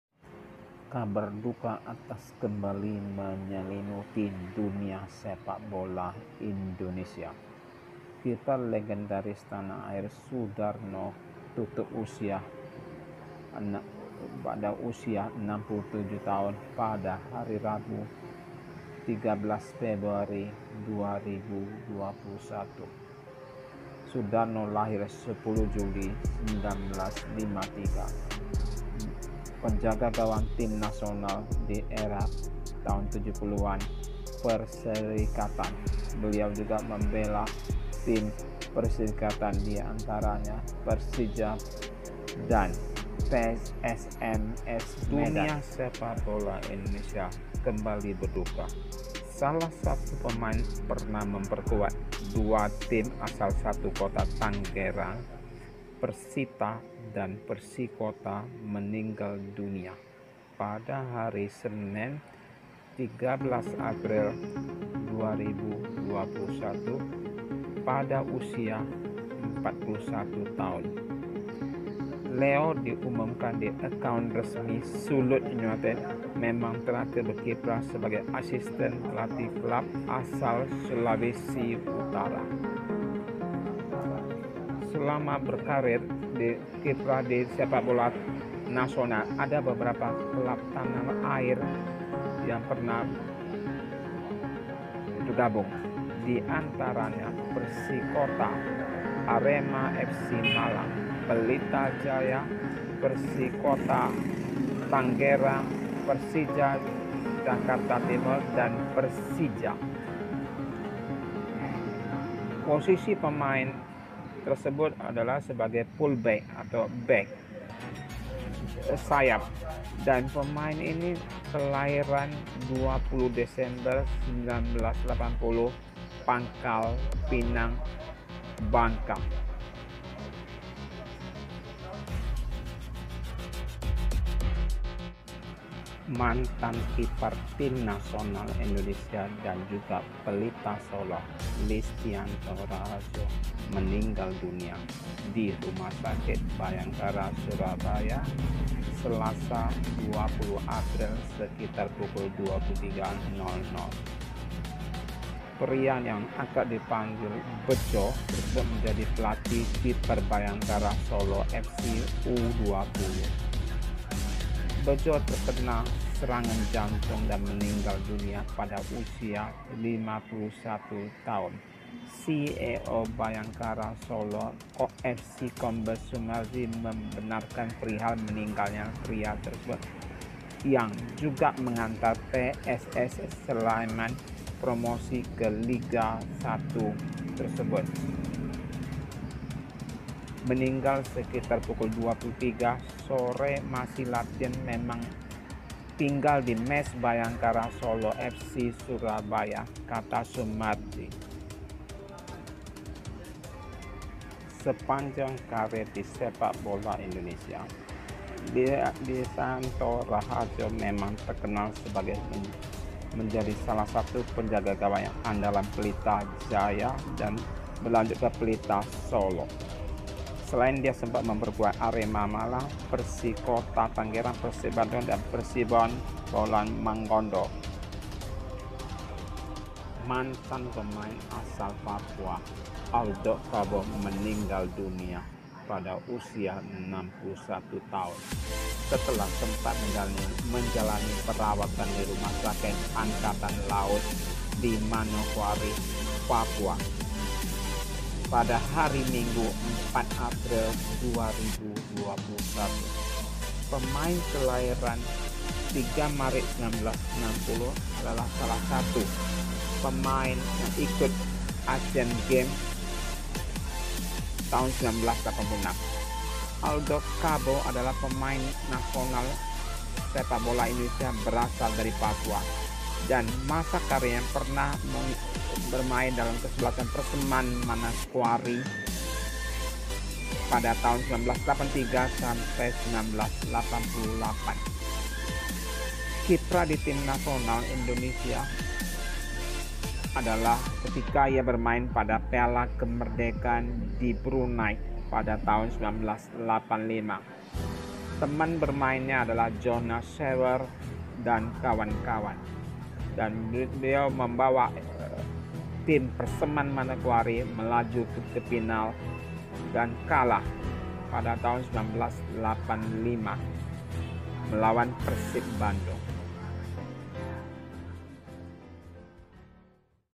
Pemain Sepak Bola Tutup usia 2021
Mantan pemain sepak bola Indonesia tutup usia tahun 2021
1.  SUDARNO
2.  LEO SAPUTAN
3.  LISTIANTO RAHARJO
4.  ADOLF KABO